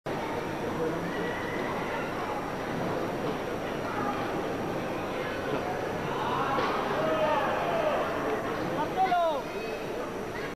Marcelo Rios MASTER VOLLEY (1998)
Marcelo Rios y una de sus clásicas voleas.

Entrenamiento 1998